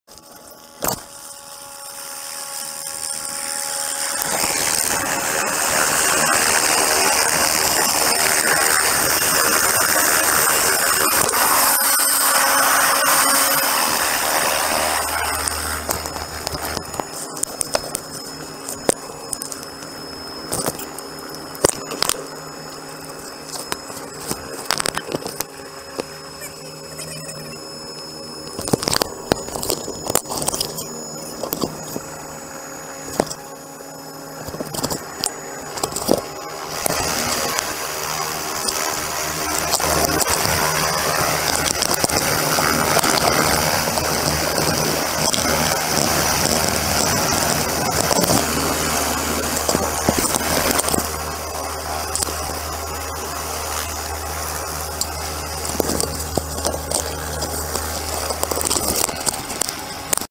Greenfields heli ops: Hot pickup x2
North Margeries Dal in Ymer Ø, east Greenland.
In this video, a rapid two pickup operation is performed. The team members are within line of sight, however they are separated by a ravine on a slope that does not permit a conventional flat surface landing.